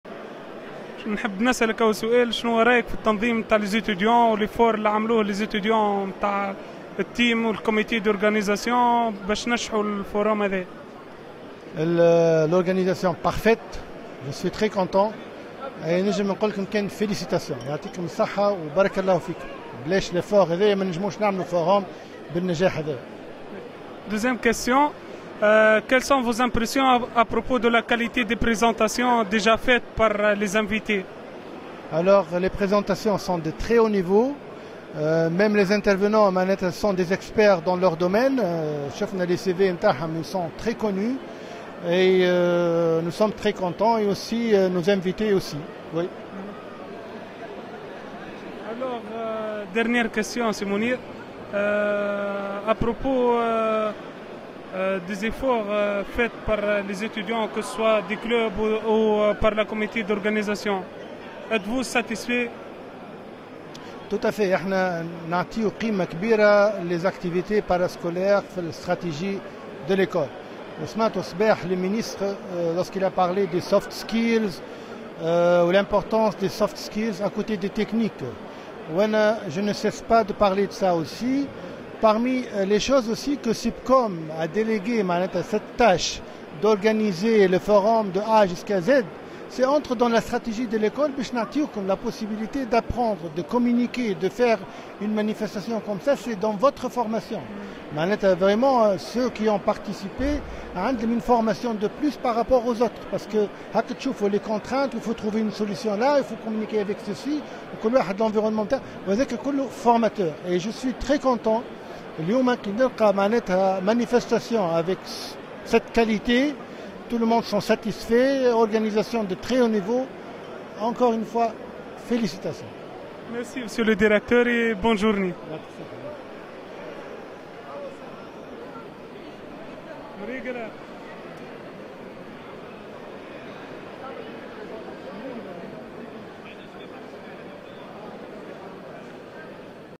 Interview avec M.Mounir Frikha
Mr. Mounir Frikha
SUPCOM director
Animé par:  ABDELJELIL Ahmed
Réalisé par: BEN ALAYA Oussama